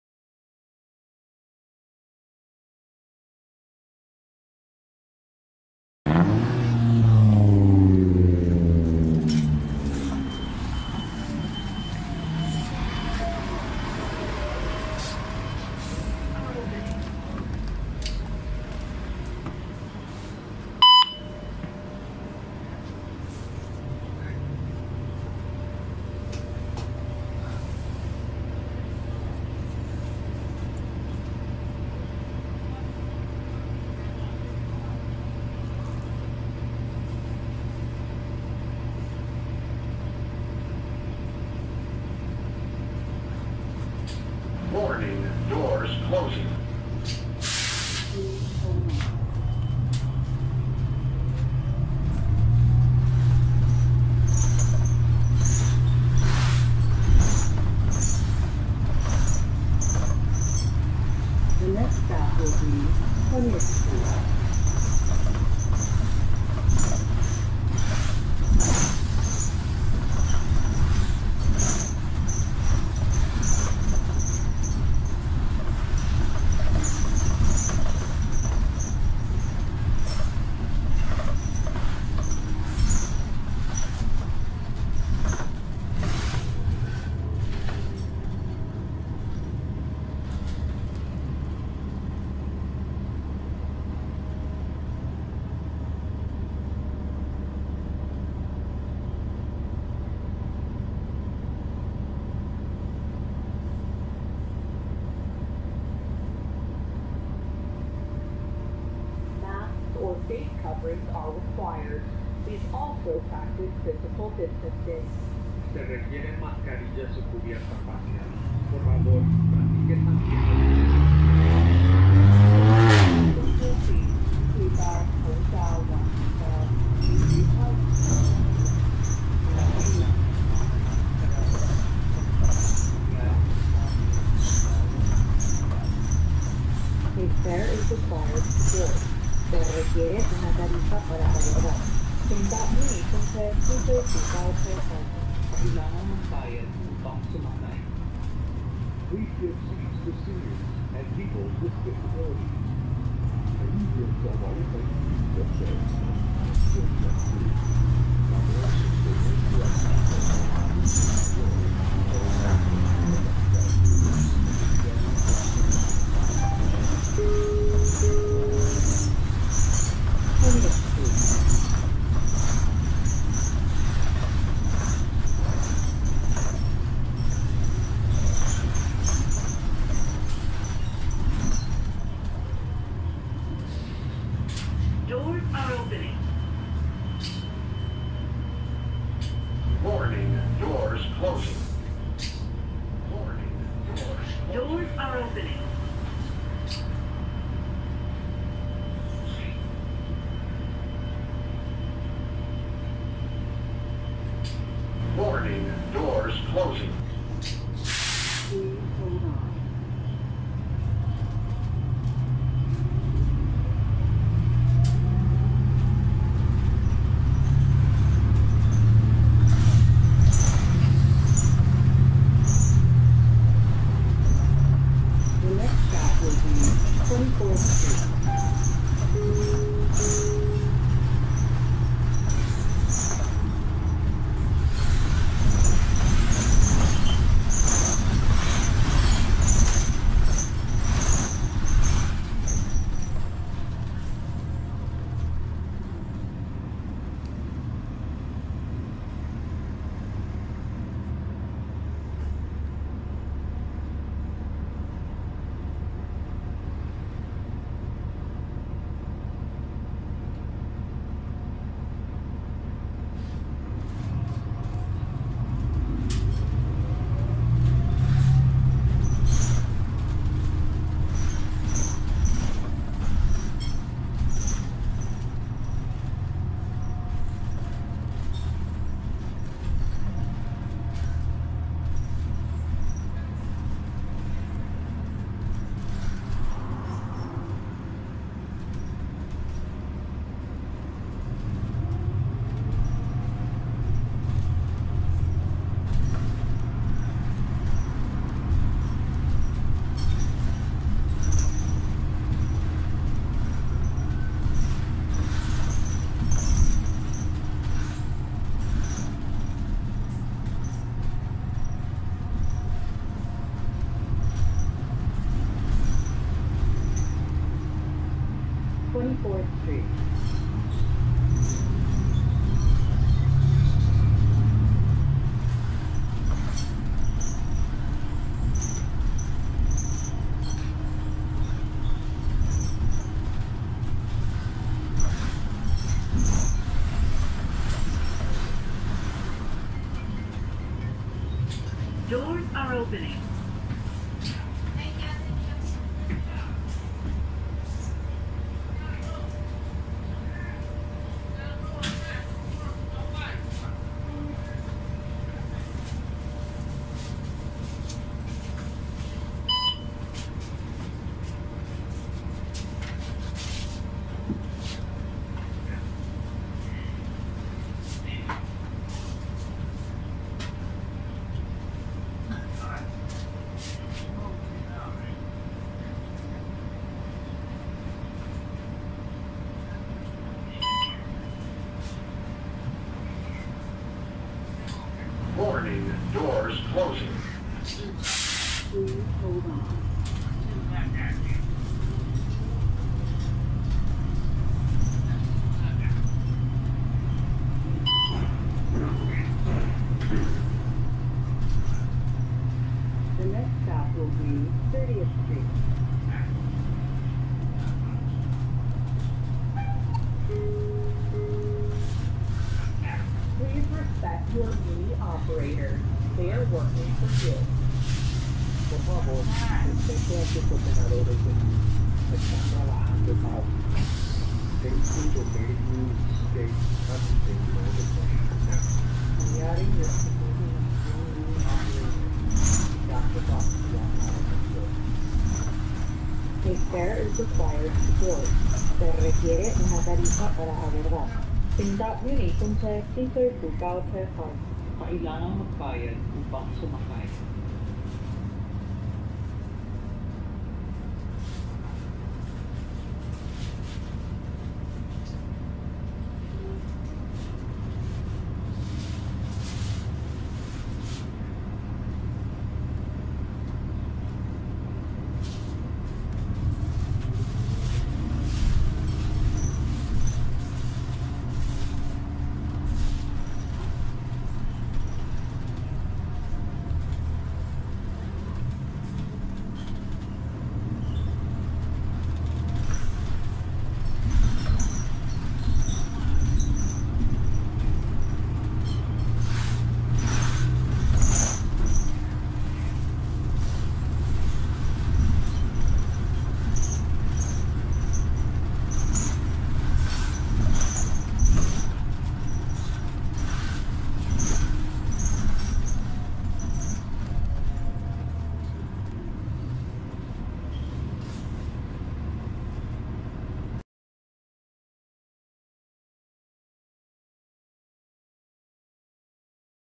SF MUNI 2018 New Flyer XDE60 #6671 on Route 14R-Mission Rapid
Recorded on January 31, 2021. This is another exclusive fragment from my next full-length film, which is almost ready. I just have to post the trailer for it.

Model: 2017 New Flyer XDE60
Engine: Cummins ISL9
Transmission: Allison H 50 EP
Division: Flynn
Route: 14R-Mission Rapid
Direction: Daly City BART Station